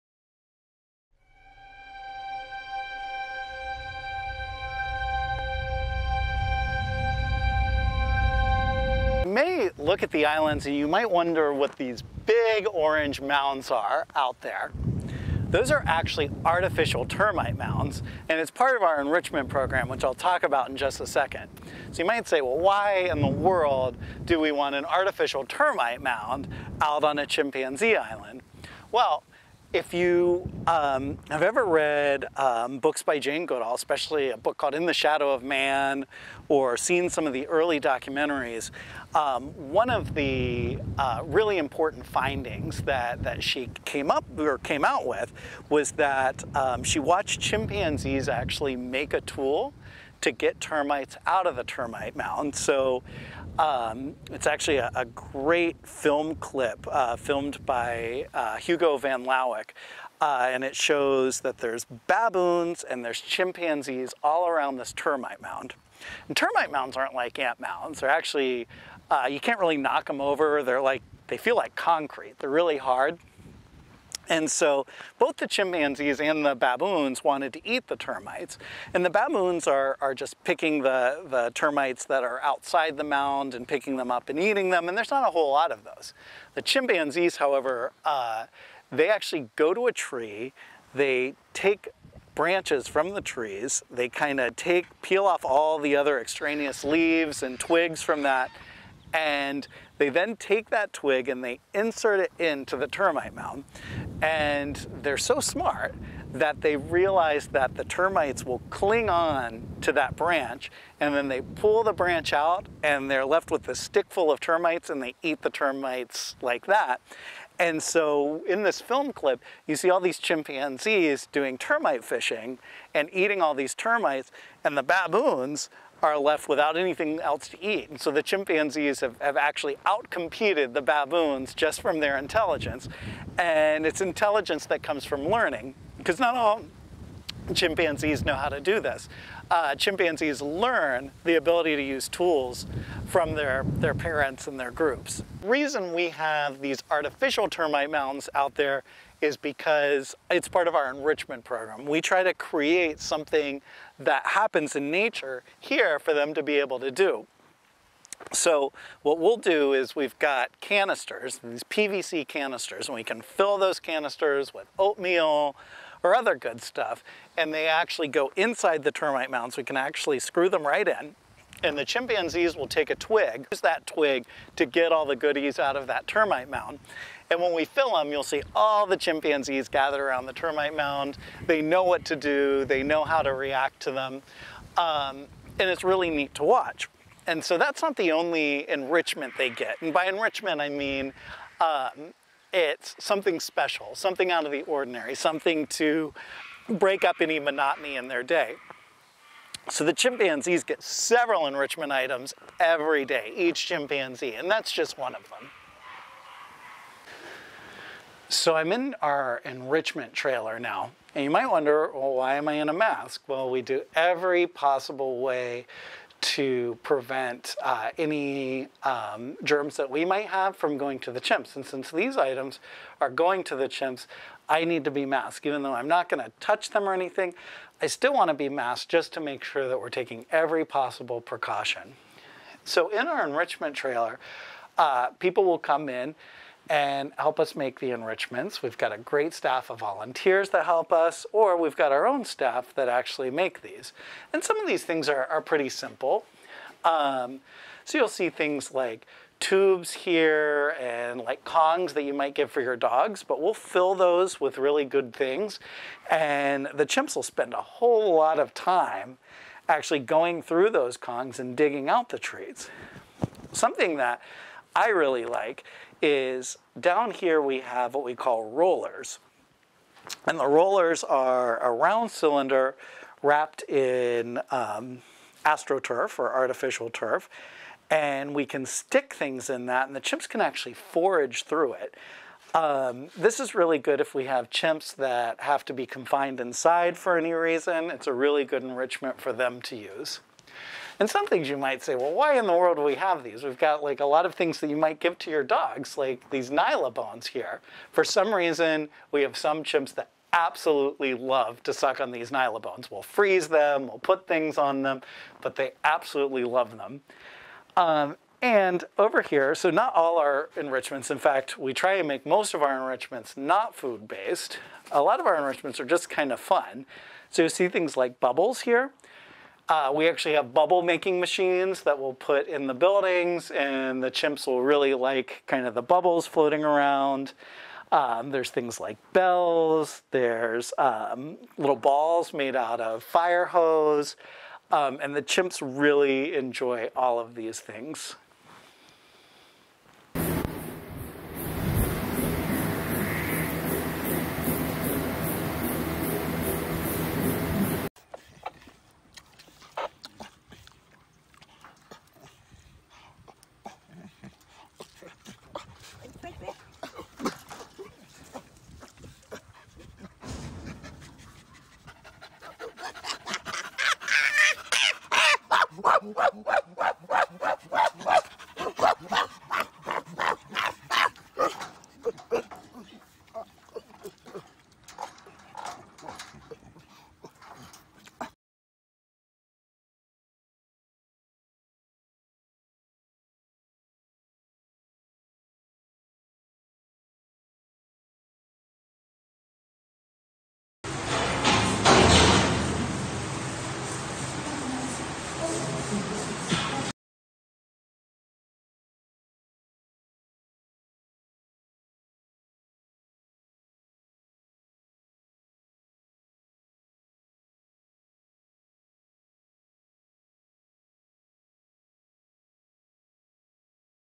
Virtual Field Trip of Save the Chimps - Episode 5
In this episode, Dr. Andrew Halloran will show you different forms of enrichment given to the chimpanzees residing at Save the Chimps.